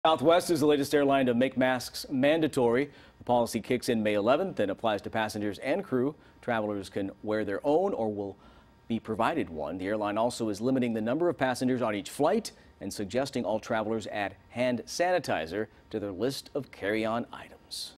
Southwest Airlines Makes Masks Mandatory
The airline is now requiring passengers to wear masks. They will provide a mask if guests don't have their own.